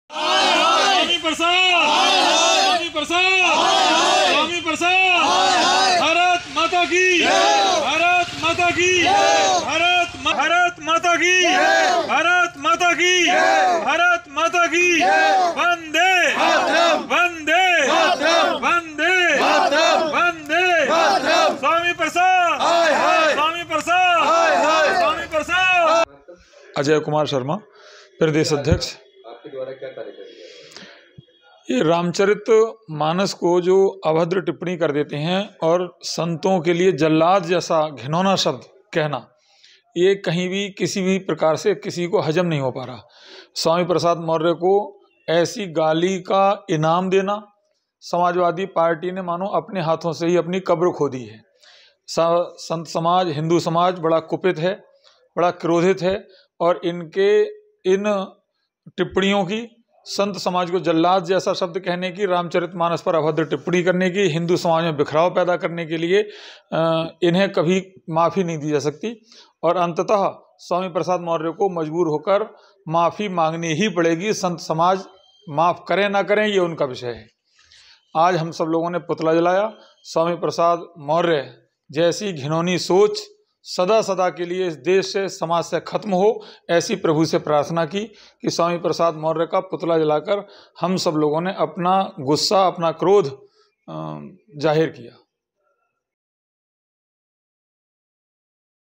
Ramcharitmanas Controversy : स्वामी प्रसाद मौर्य के पुतले को पहनाई जूते चप्पलों की माला, फूंका पुतला
संभल में हिंदू जागृति मंच के कार्यकर्ताओं ने स्वामी प्रसाद मौर्य को राक्षसी प्रवृत्ति का नायक बताया. साथ ही स्वामी प्रसाद मौर्य का पुतले को जूते की मासा पहनाकर चप्पलों से पिटाई की.